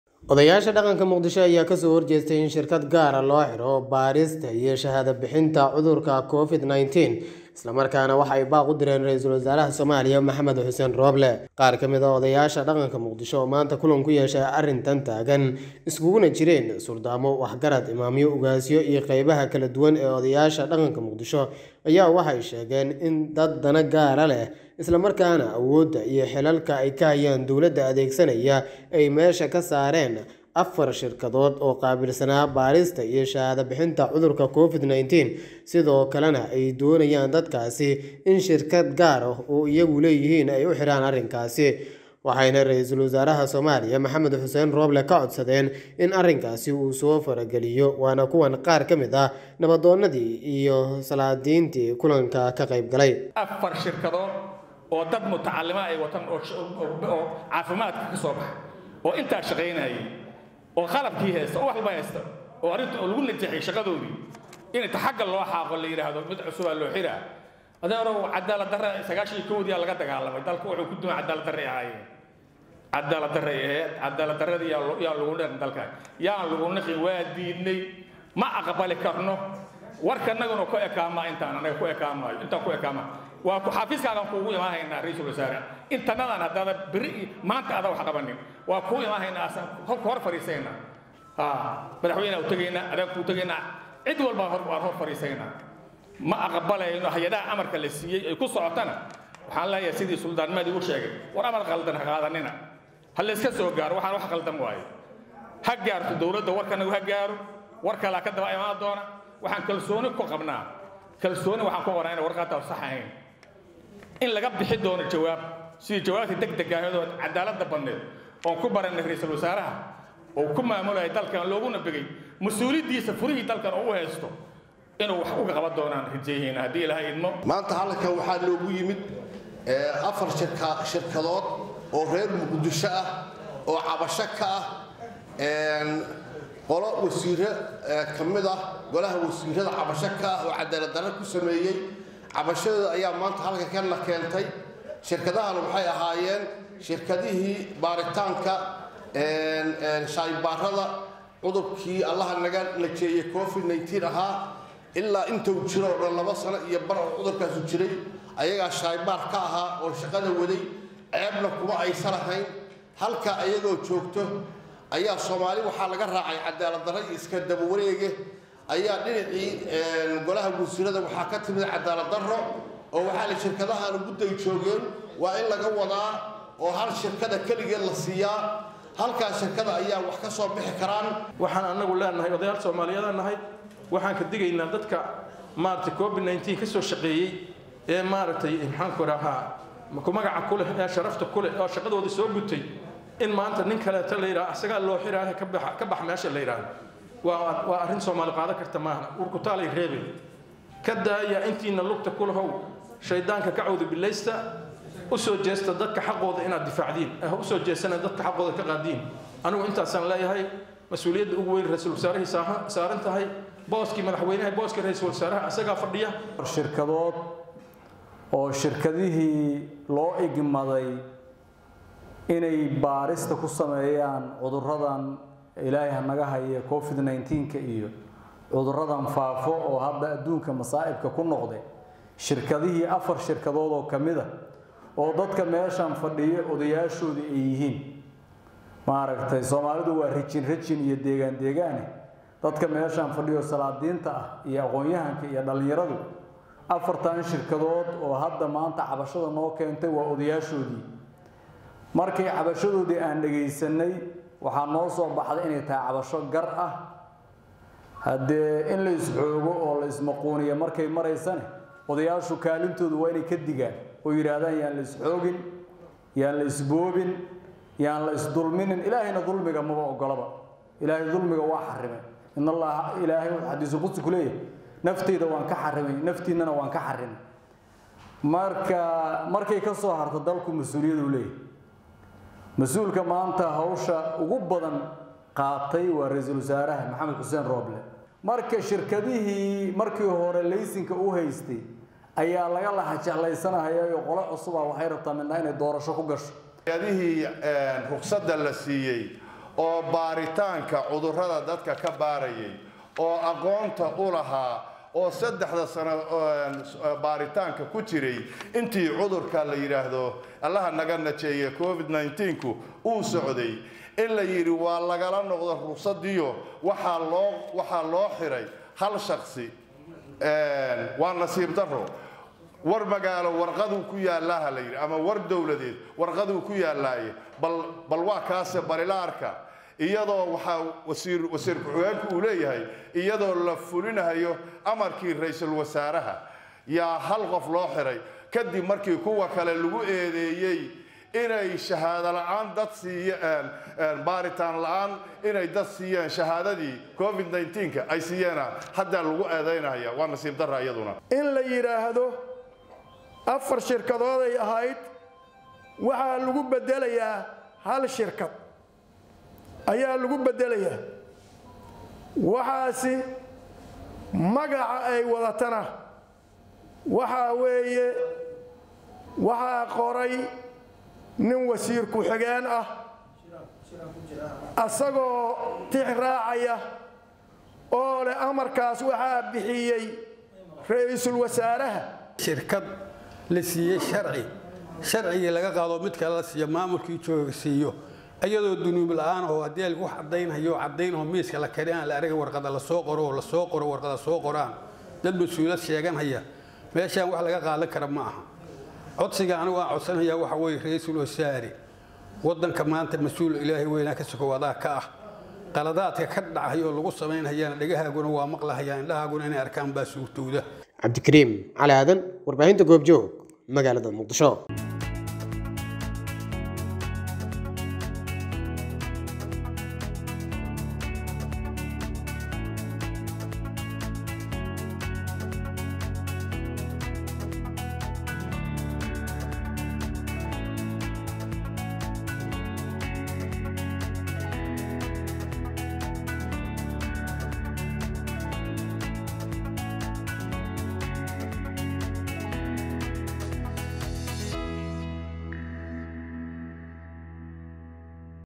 Odayaasha dhaqanka Muqdisho oo ka soo horjeestay in shirkad gaar ah loo xiro baarista Covid-19
Odayaasha dhaqanka Muqdisho oo ka soo horjeestay in shirkad gaar ah loo xiro baarista Covid-19 isla markaana baaq u diray Ra'iisul Wasaare Rooble.